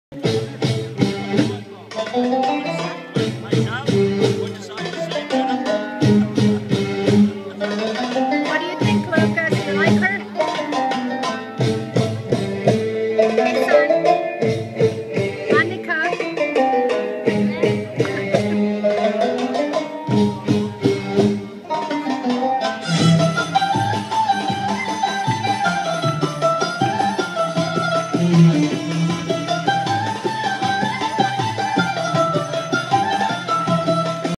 Belly dancing
After a fabulous BBQ buffet dinner with unlimited wine, the crowd was mesmerized by our beautiful belly dancer.